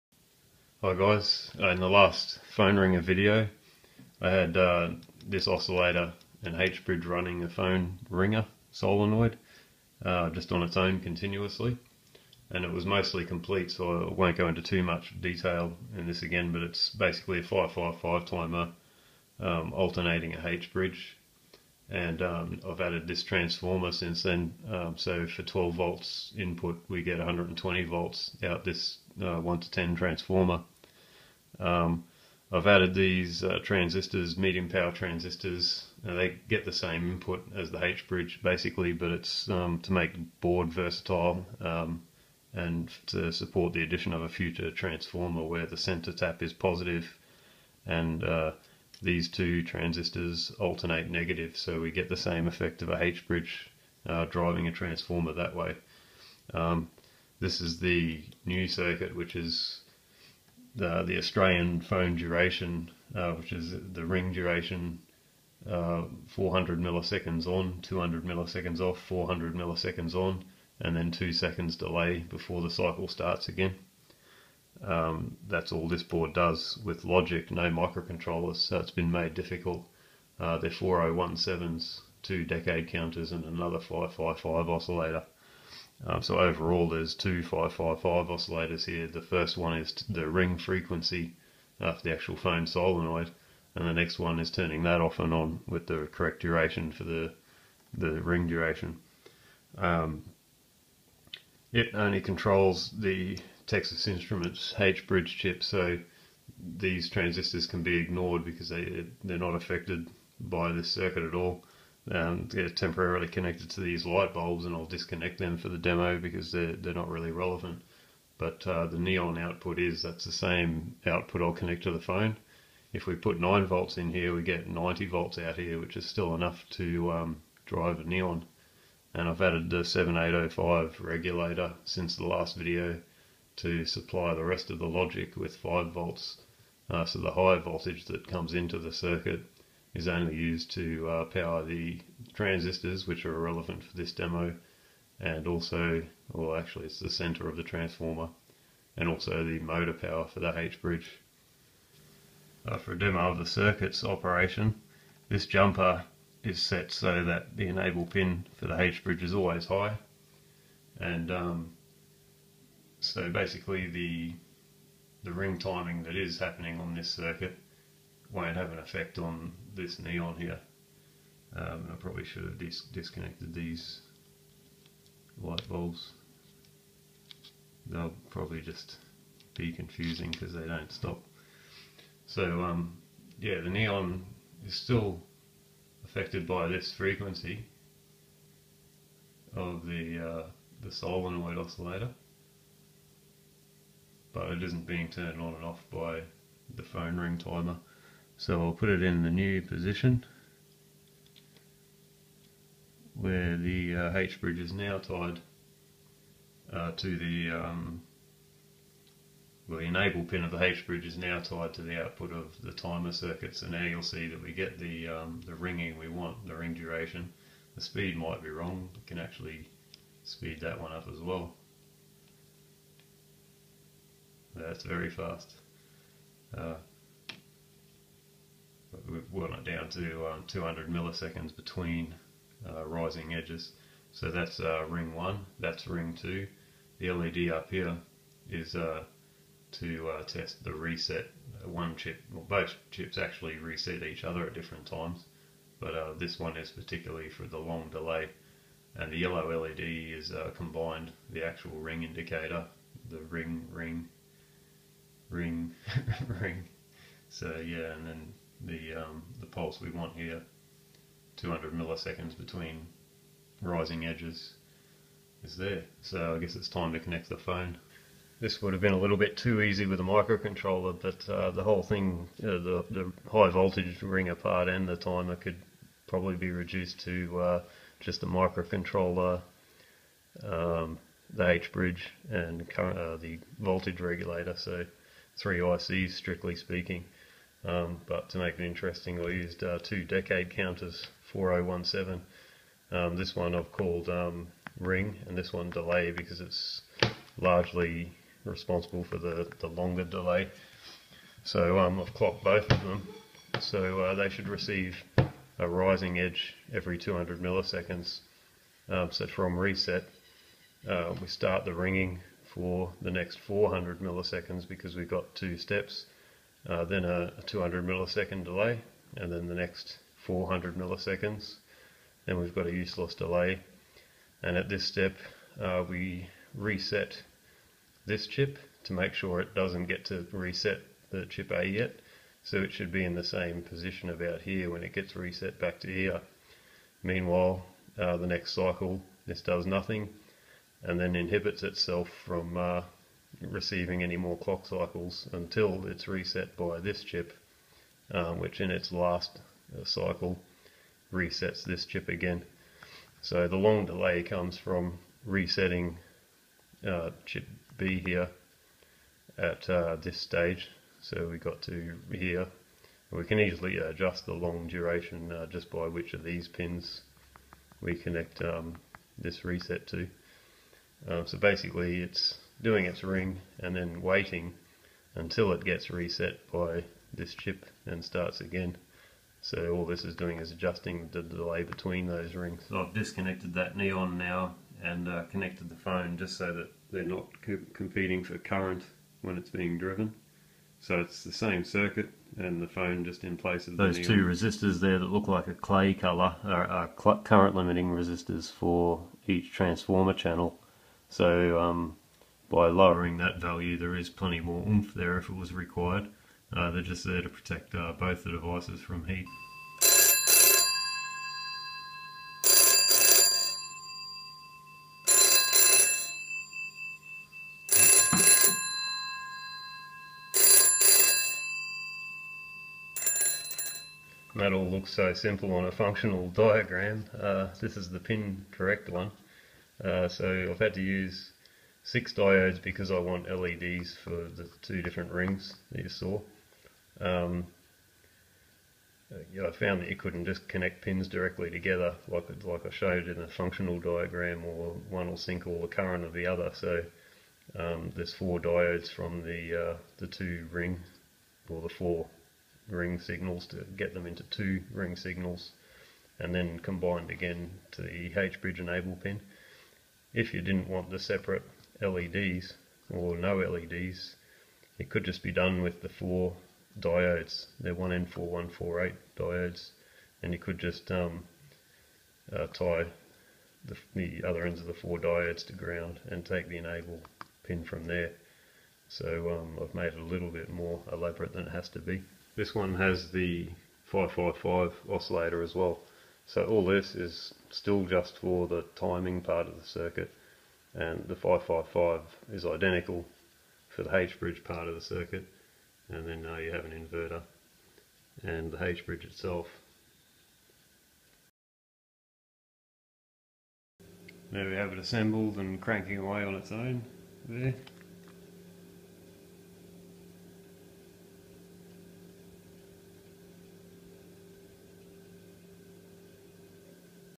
Australian Telephone Ringer Part 2
Hi Guys :) An attempt to simulate Australian standard phone ringer with old school digital logic.